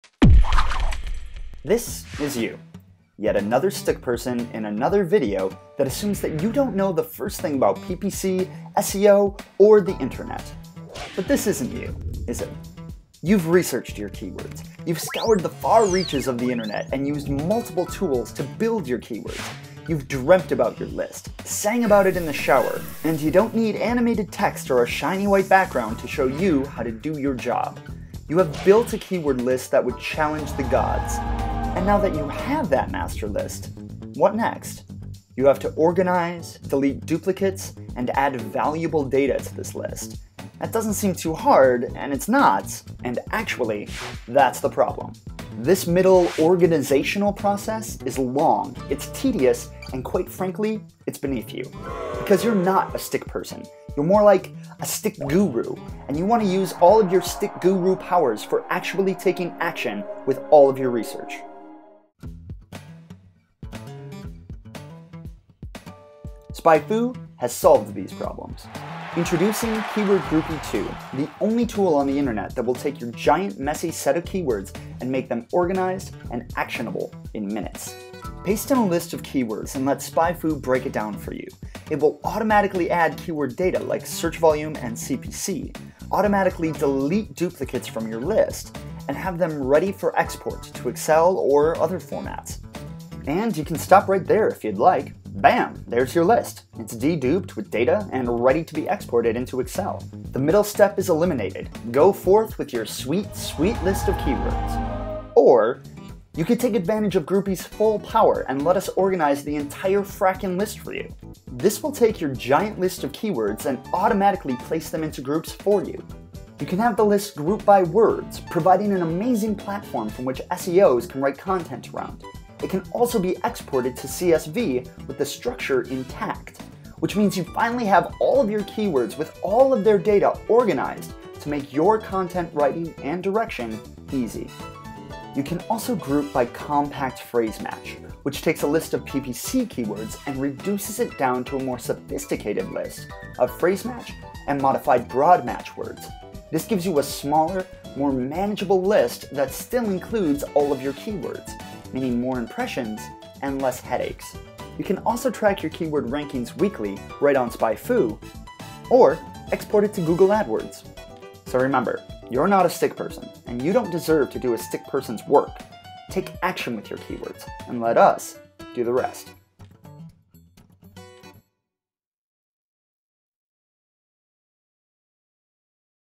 Become a Keyword Groupie | SpyFu Keyword Groupie
Let us take your list of keywords and automatically create your Ad Groups and a platform for your SEO Content